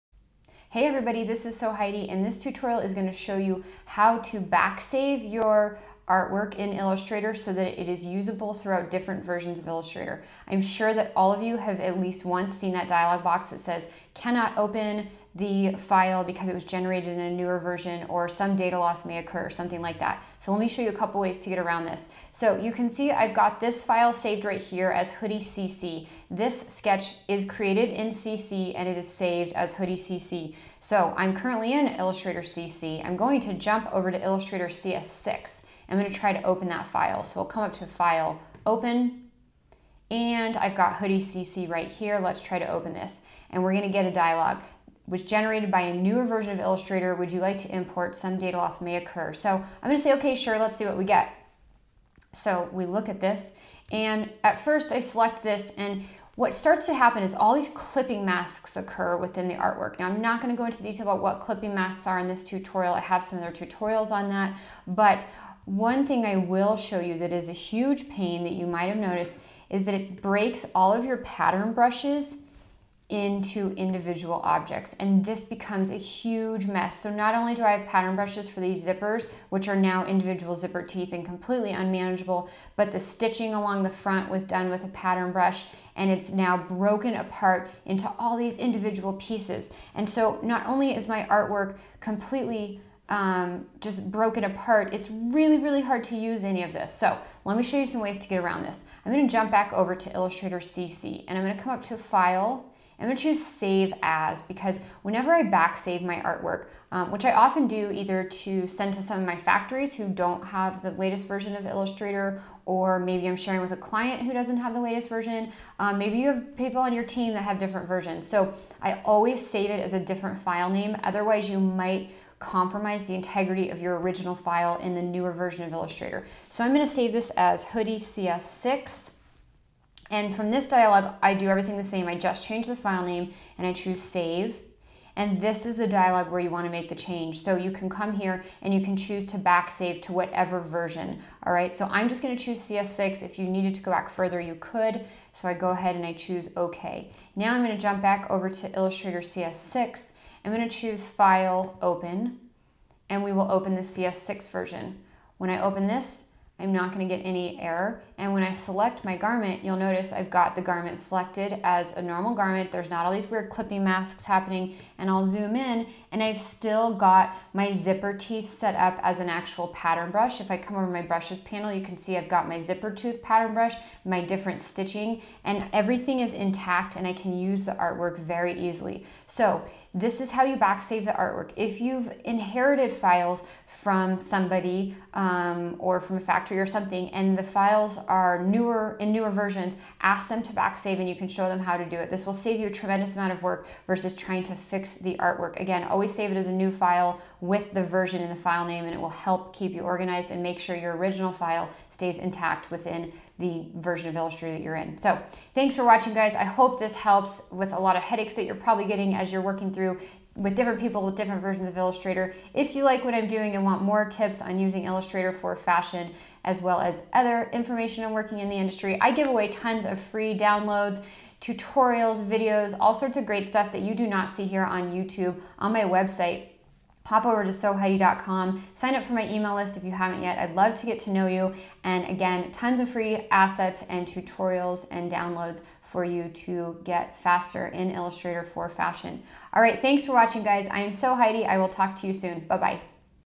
How to Back Save Newer Illustrator files to Open in Older Versions
You've probably seen one of these error messages before:

"The file was generated by a new version of Illustrator. Would you like to import this file? Some data loss may occur."

"Can’t open the illustration. The file was generated by a newer version of Illustrator and cannot be opened. Please resave it in a format compatible with your version of Illustrator."

This tutorial will show you how to prevent them so your artwork is actually useable (and all of your brushes, pattern swatches and artwork don't break apart).

You will learn:

00:22 What happens when you open a newer AI file in an older version of Illustrator
01:40 Best practices and how to save an Illustrator file as an older version so other users can open the file